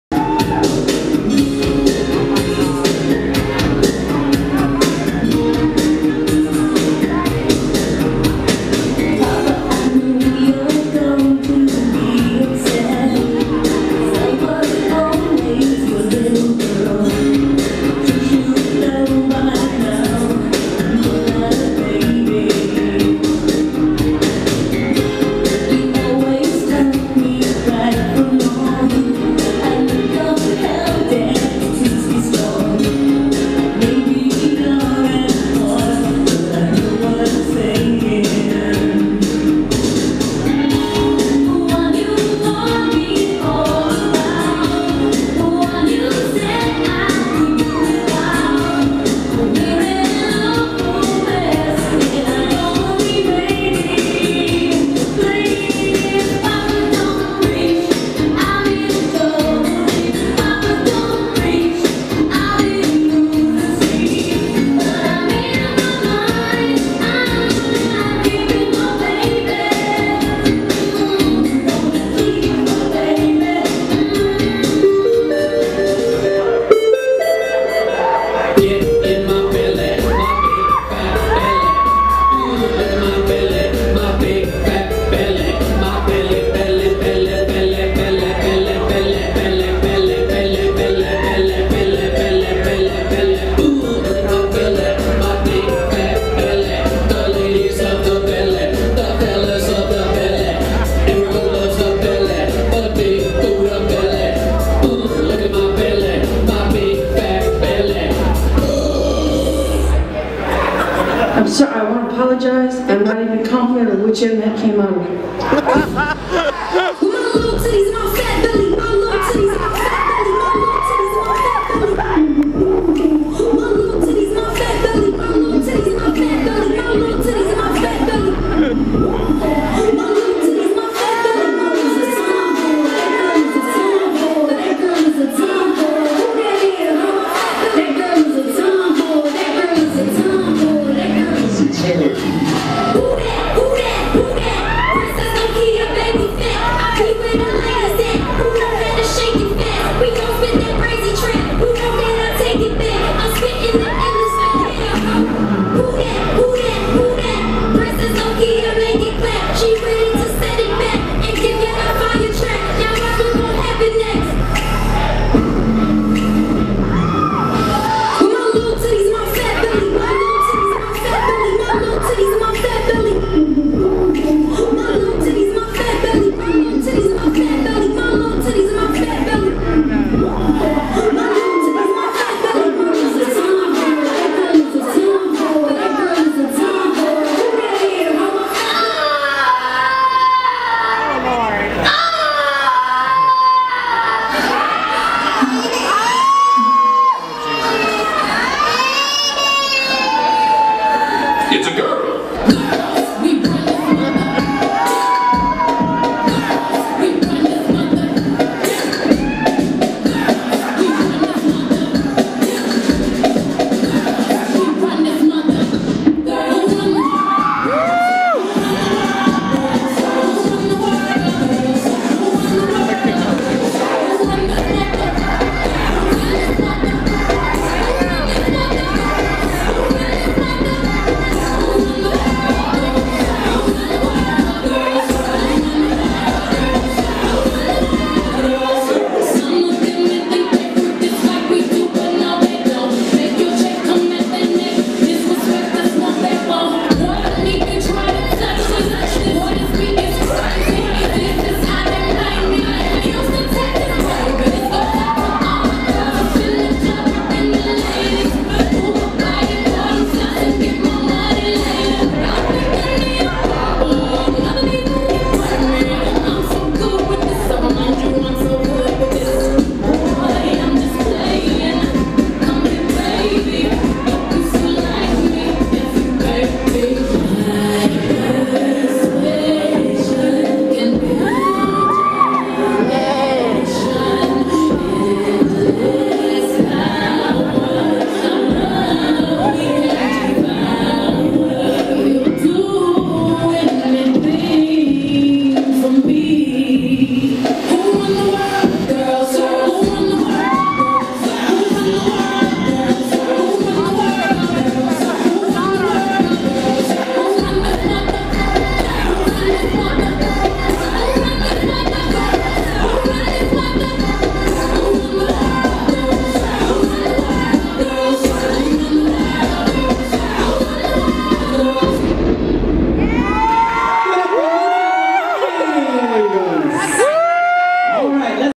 Luna of the Lilies @ Rich's Houston Nightclub
Luna of the Lilies performing at Rich’s Dessie's Drag Race: Season 14  in Houston! - June 4, 2018